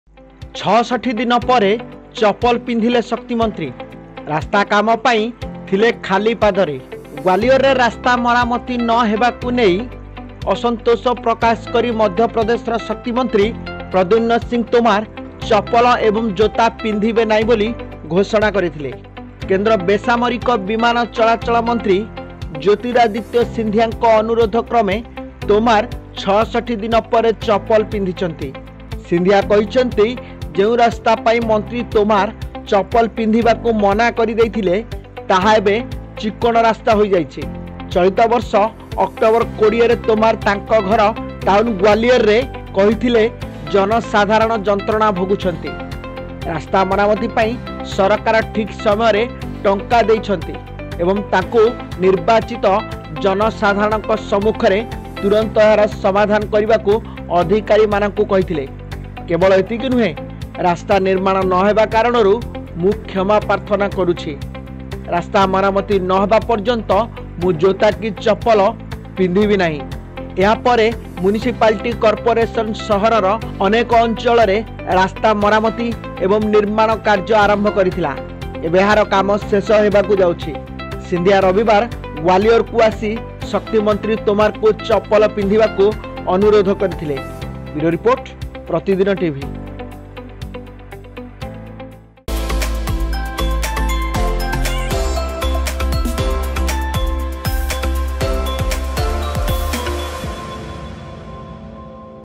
ସରିଲା ରାସ୍ତା କାମ, ୬୬ ଦିନ ପରେ ଚପଲ ପିନ୍ଧିଲେ ଶକ୍ତି ମନ୍ତ୍ରୀ !| Scindia helps MP minister wear slippers
ସରିଲା ରାସ୍ତା କାମ, ୬୬ ବର୍ଷପରେ ଚପଲ ପିନ୍ଧିଲେ ଶକ୍ତି ମନ୍ତ୍ରୀ ! |  Scindia helps MP minister wear slippers